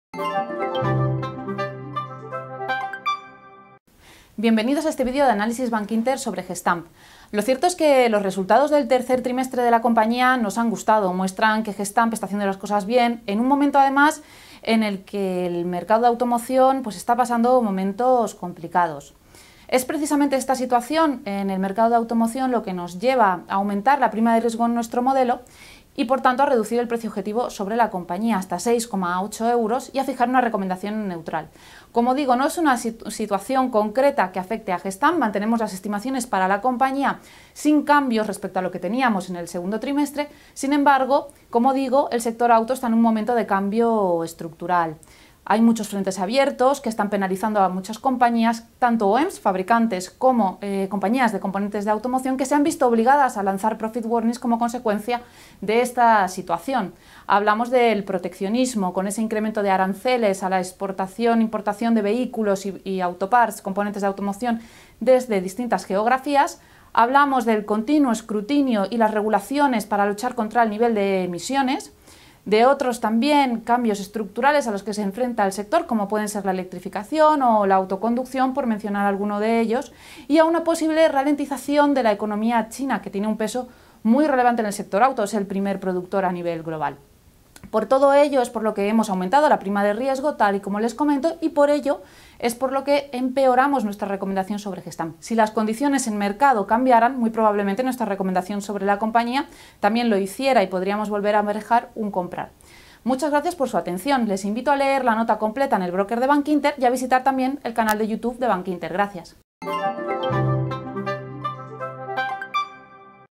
Análisis Gestamp por Esther Gutiérrez de la Torre, analista de Bankinter
Análisis resultados tercer semestre 2018 de Gestamp (19 de noviembre de 2018)